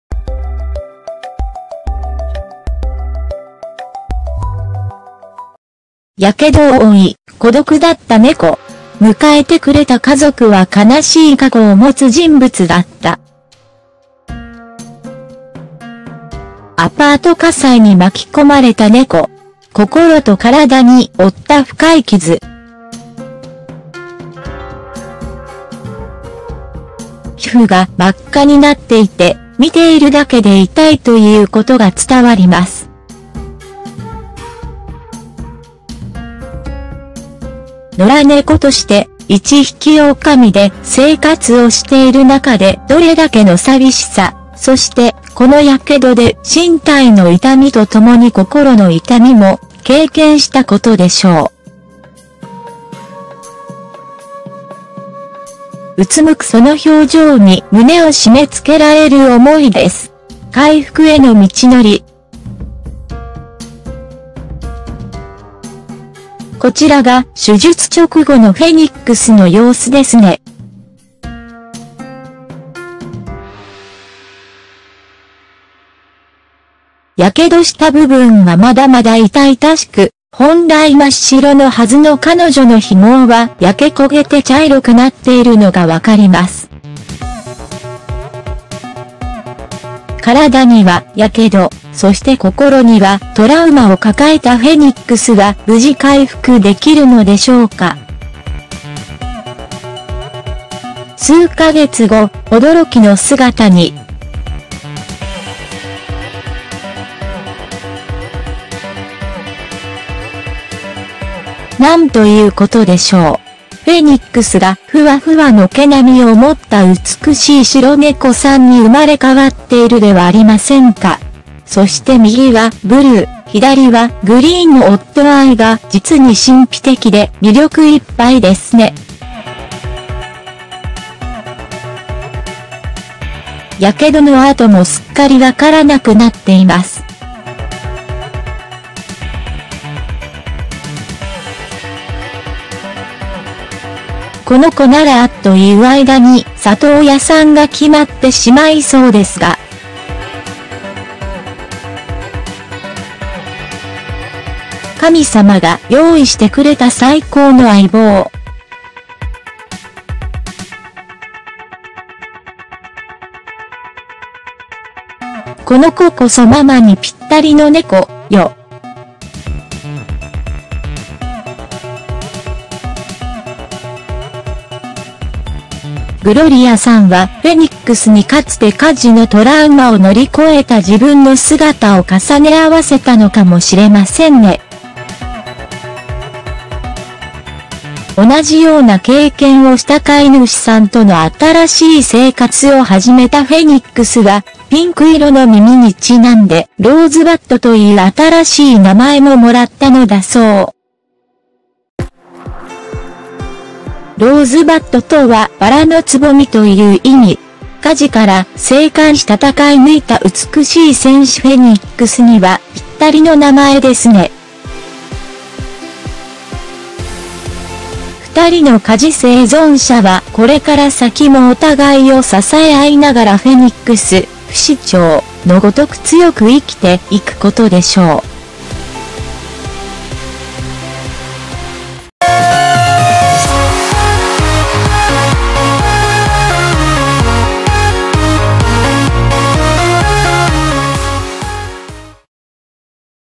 火傷を負い孤独だった猫。迎えてくれた家族は悲しい過去を持つ人物だった - Japan Pets
猫のフェニックスは火事に遭い火傷を負っていたところを助けられ、コロラド州デンバーにある動物保護団体Dumb Friends League(DFL)にレスキューされました。やがて辛い治療を乗り越えたフェニックス。そんな彼女に待っていたのは意外な人物との出会いだったのです。
Kawaii Pet TV